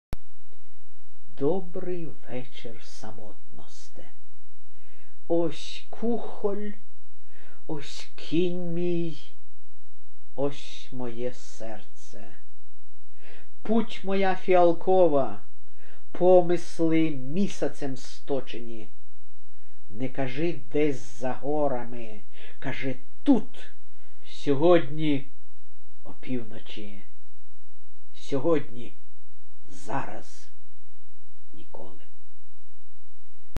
Емма Андієвська, Добрий вечір, самотносте
Емма Андієвська читає свій вірш "Добрий вечір, самотносте".
Emma Andijewska reading her poem "Dobryi vechir, samotnoste" in Ukrainian.